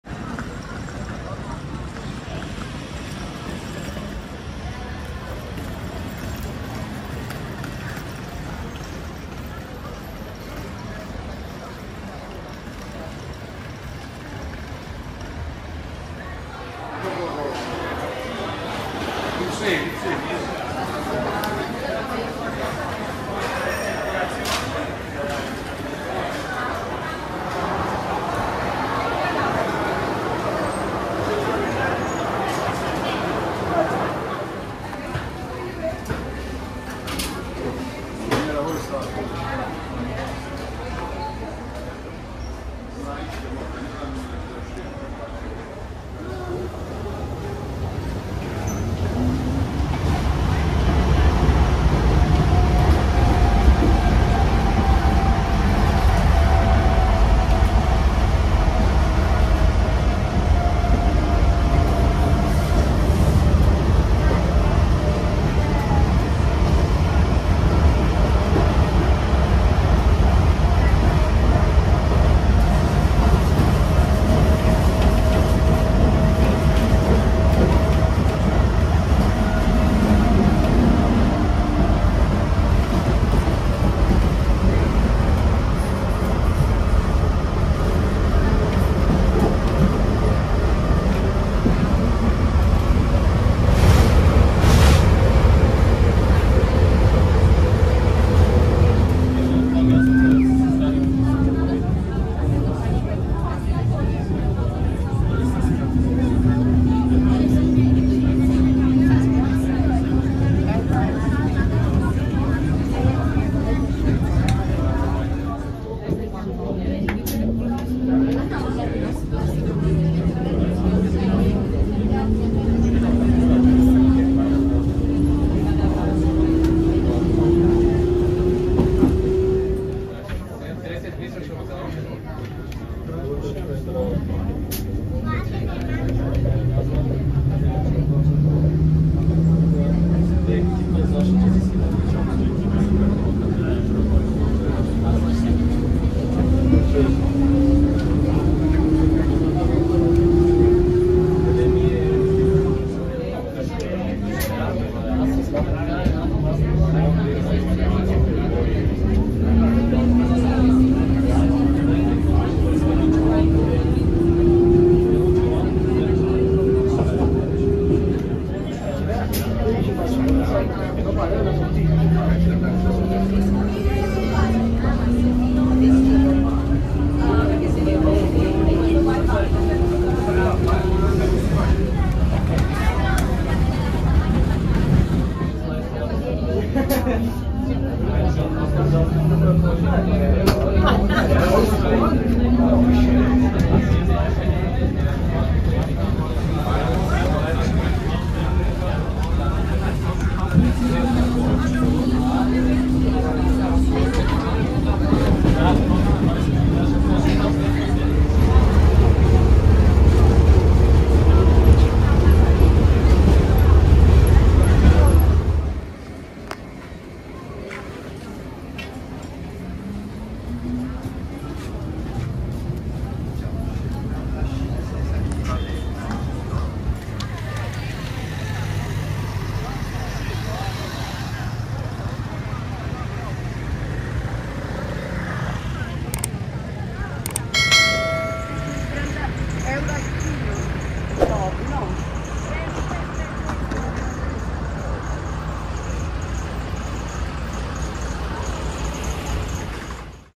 How to go to Vico Equense from Naples by Train 🇮🇹 4K UHD
ALL RAILWAY STATIONS - Naples to Vico Equense

🔘GaribaldiStation

🔘Torre Annunziata
🔘Stazione Villa Regina
🔘Pompei Scavi Villa Dei Misteri
🔘Pioppaino
🔘Via Nocera
🔘Castellammare di Stabia

🔘Vico Equense